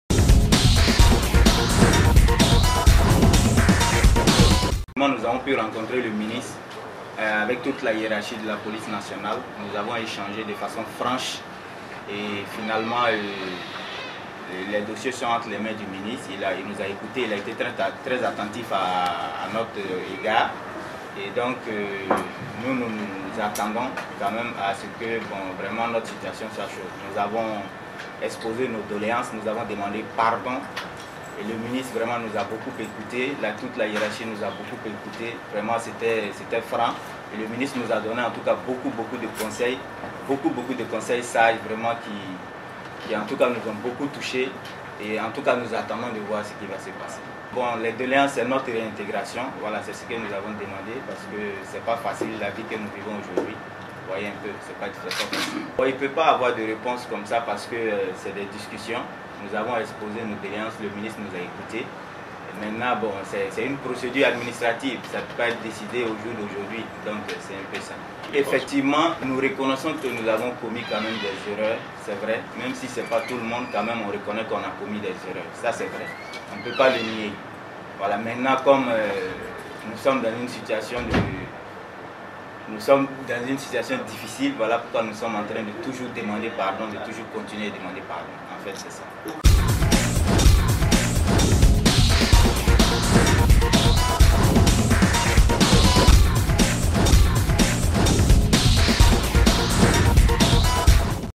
Simon Compaoré et policiers radiés : «Nous avons échangé de façon franche», Omar Junior Bahoro
Omar Junior Bahoro, le rapporteur général des policiers radiés fait le point de la rencontre avec le ministre d'Etat, ministre de l’Administration territoriale, de la décentralisation et de la sécurité intérieure (MATDSI), Simon Compaoré le mardi 10 mai 2016.